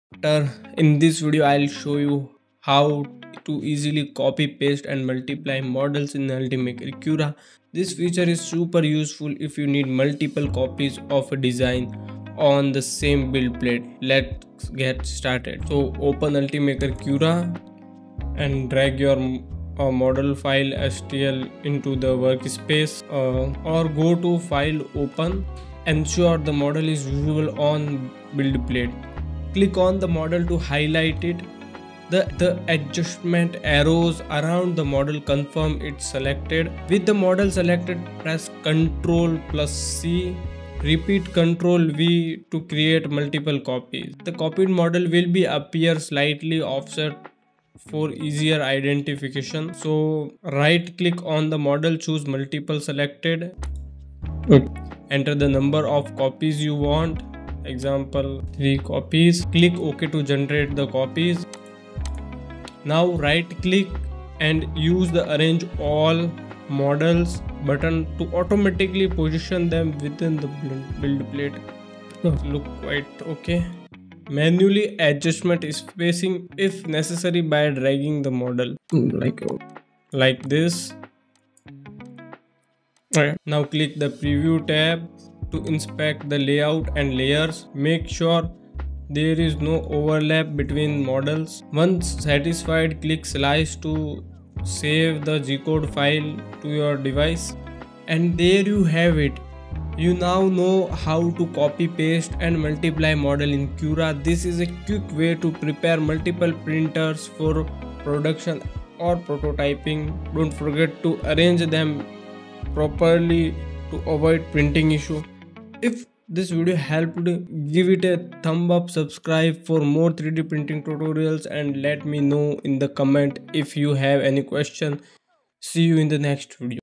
How to Duplicate or Multiply Models in Ultimaker Cura | Easy Step-by-Step Guide
Learn the fastest way to copy, paste, and multiply models in Ultimaker Cura! In this beginner-friendly tutorial, I'll walk you through step-by-step how to duplicate your designs on the build plate, optimize their placement, and prepare them for 3D printing. Save time and boost productivity with these easy-to-follow tips.
Learn how to copy, paste, or multiply models in Ultimaker Cura with this easy step-by-step tutorial. Whether you're a beginner or an experienced user, this guide will help you optimize your 3D printing workflow and save time. Watch now to master the art of duplicating models effortlessly in Cura. Perfect for anyone interested in 3D printing!

What You'll Learn:
✔️ How to copy and paste models in Cura.
✔️ How to multiply models for 3d printing.
✔️ Tips to arrange and optimize models on the build plate.

Chapters:
0:00 – Welcome! Why Duplicating Models in Cura is a Game Changer
0:15 – Step-by-Step: How to Easily Copy & Paste Models in Cura
0:50 – Step-2 choose Multiply select
1:04 – Maximizing Efficiency: Using the Multiply Tool for Bulk Printing
1:28 – Perfecting Your Layout: Tips for Optimizing Model Placement